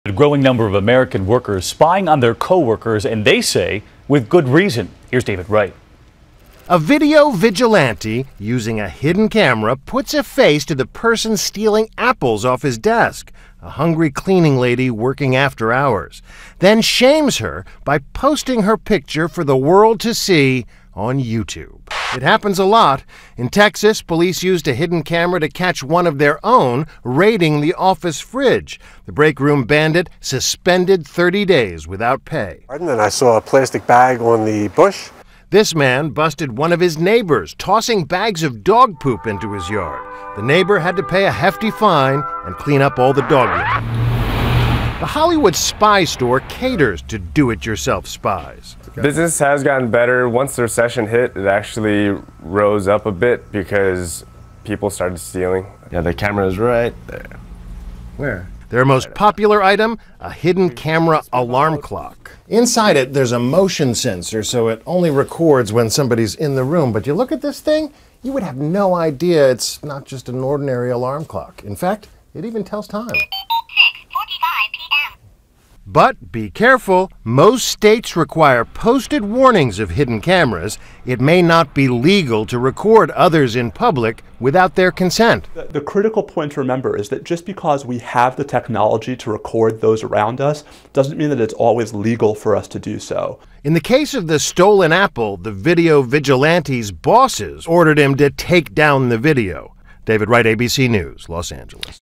Man Uses Webcam to Catch Cleaning Lady Stealing
Growing number of Americans use hidden cameras to spy on co-workers.